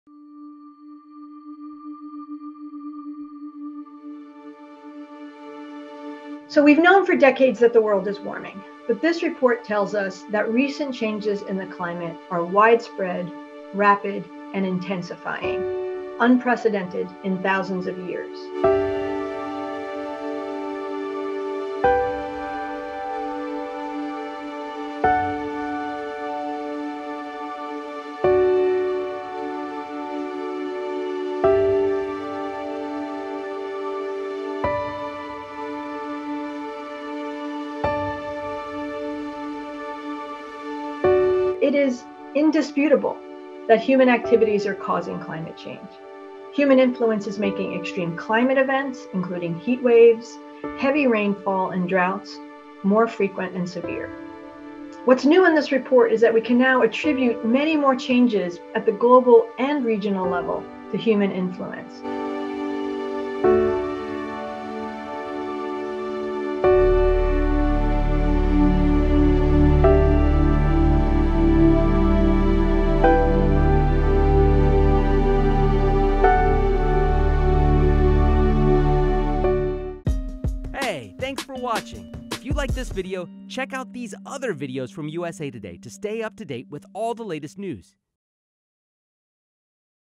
New climate change report from the IPCC issues 'code red for humanity' | USA TODAY
Top scientists have released a devastating report on the danger climate change poses to the world.

The United Nations' Intergovernmental Panel on Climate Change releases a new and devastating report on the danger climate change has on the world.